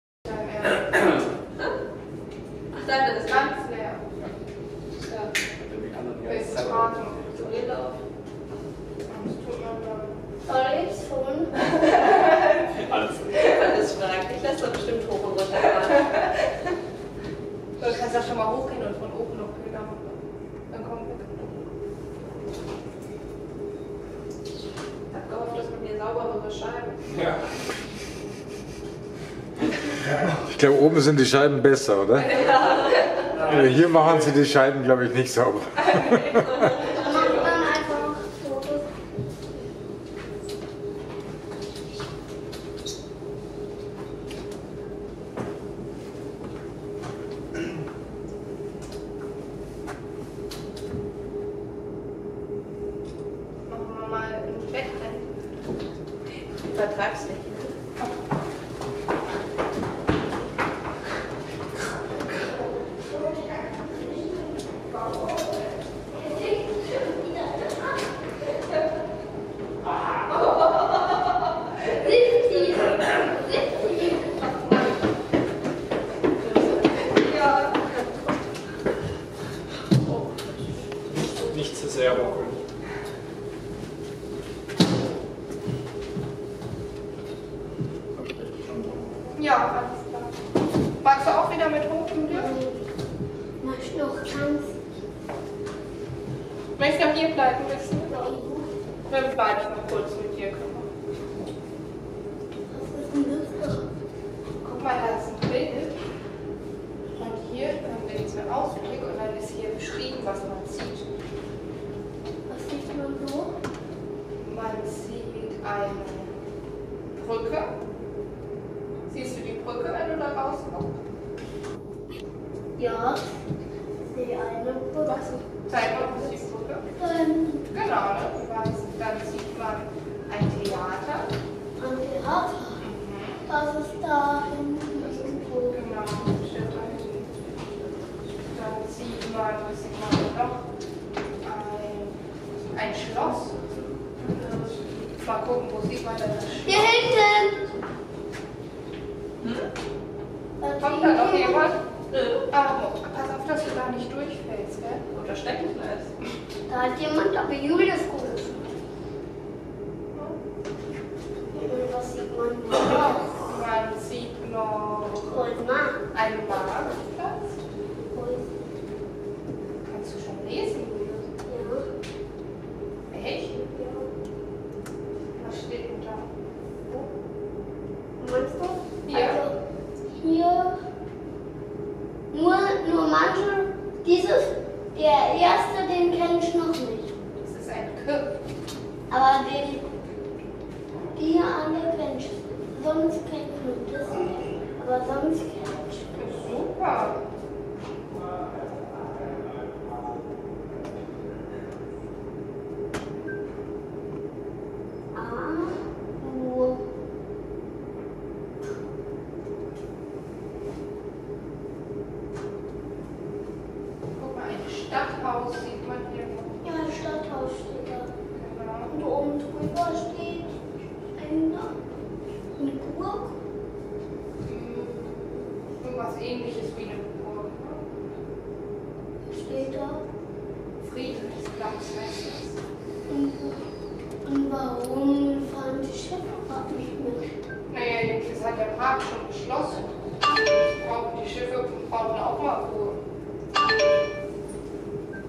Sonnenuntergang Lumix fz82 4K 14.09.2019
Gefilmt vom Fernsehturm Mannheim.
Leider nicht besser möglich.leider schmutzige Fenster und Fenster davor.
Panasonic Lumix FZ82 mit DMW LT55